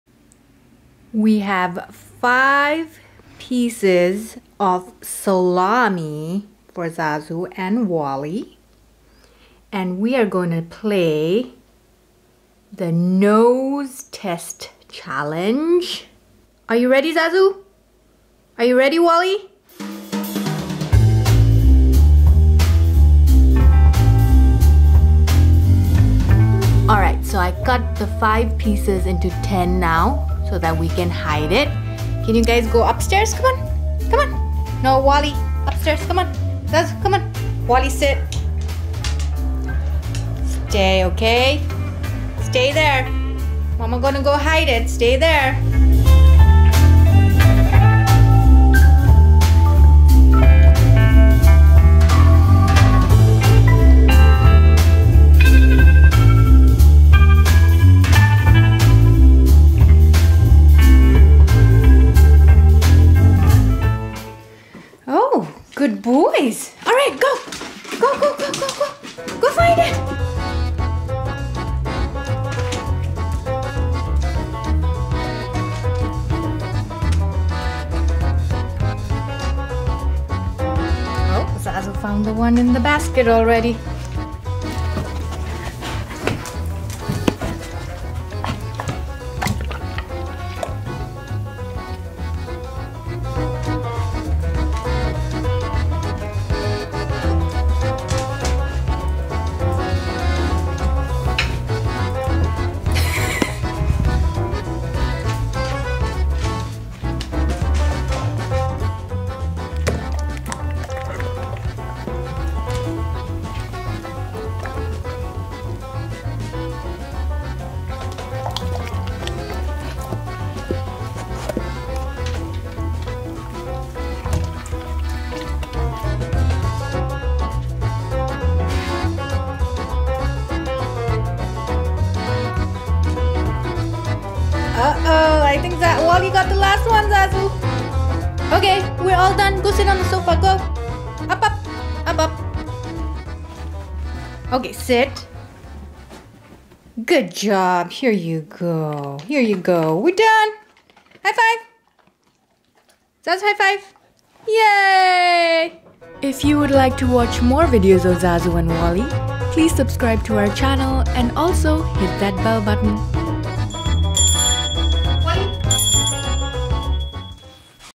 Nosework Is the Secret to a Calm Happy Dog
How we keep Zazu and Walle busy

"With nose work you're using your dog's natural talent in a constructive way to keep them mentally stimulated. Dogs thrive when they feel like they have a job to do and it's even better when it's something as simple as teaching them to sniff things out for you."